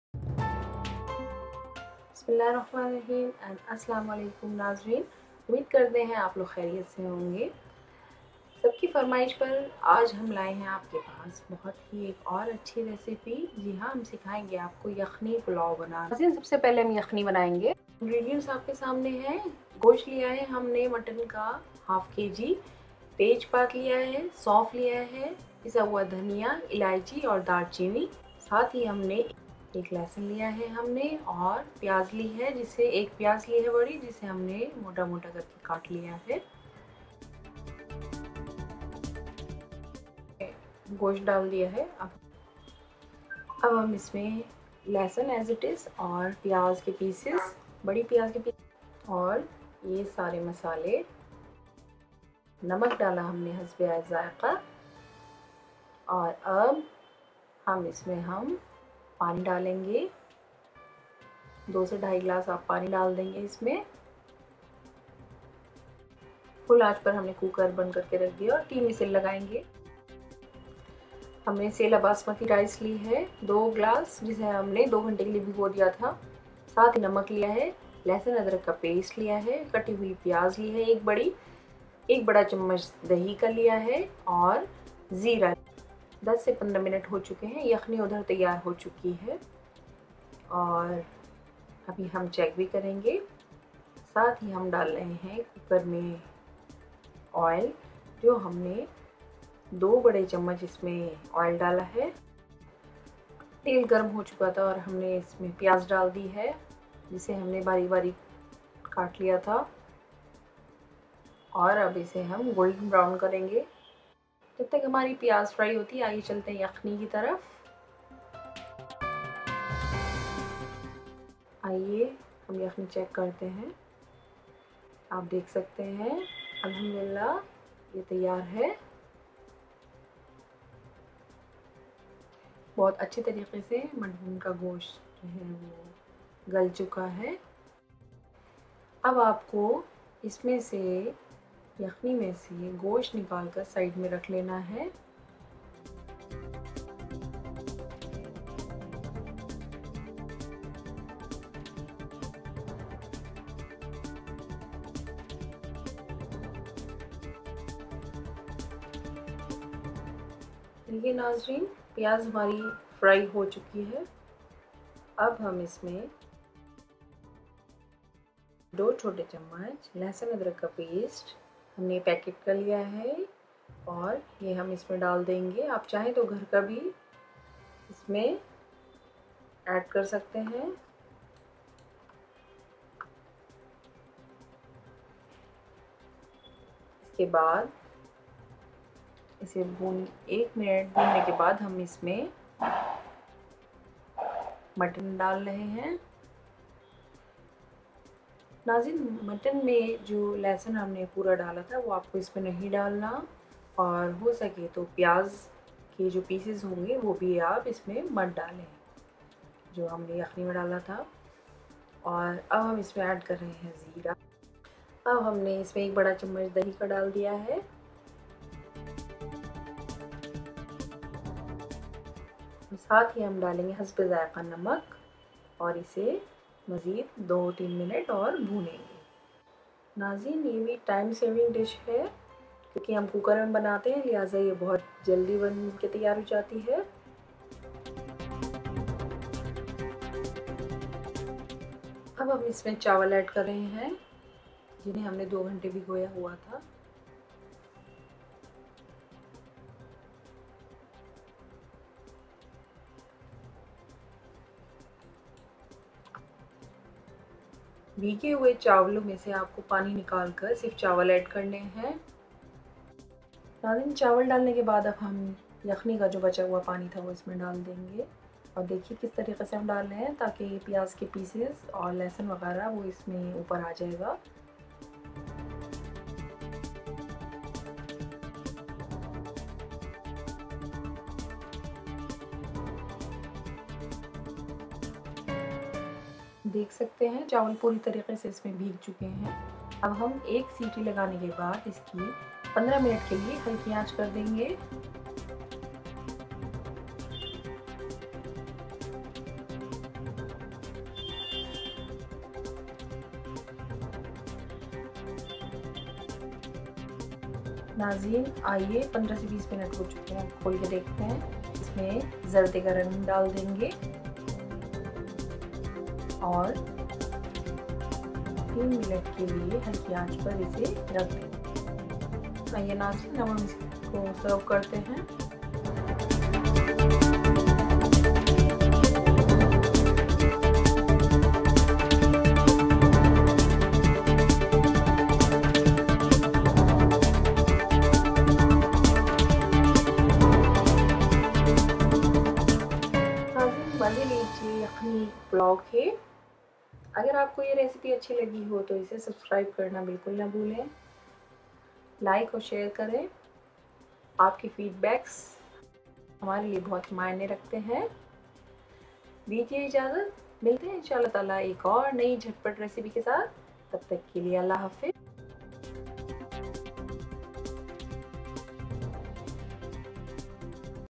How to make mutton yakhni pulao || mutton yakhni pulaao in cooker in Hindi by lazeez pakwaAns
A VERY SIMPLE , EASY AND QUICK RECIPE OF MUTTON PULAAO .IF YOU LIKE MY RECIPE PLZ SUBSCRIBE ,LIKE AND SHARE ... GIVE A TRY TO THIS RECIPE AND GIVE YOUR FEED BACKS... THANKS ... :)